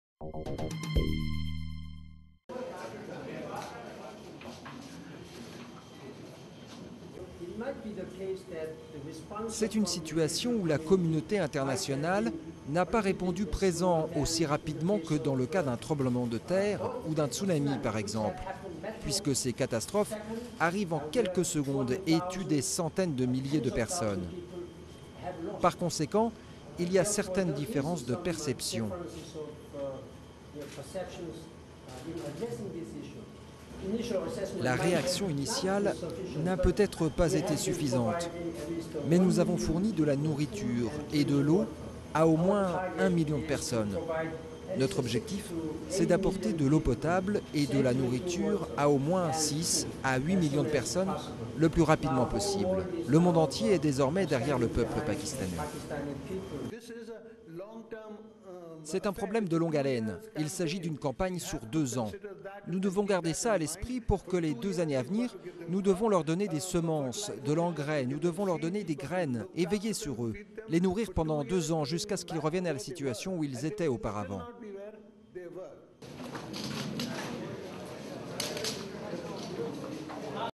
Ban Ki-moon demande au monde d'accélérer son aide au Pakistan
Le secrétaire général de l'ONU, Ban Ki-moon, est arrivé dimanche 15 août au Pakistan, où il a demandé à la communauté internationale d'accélérer l'arrivée de son aide aux 20 millions de victimes des inondations, la pire catastrophe naturelle de l'histoire du pays. Durée: 1min20.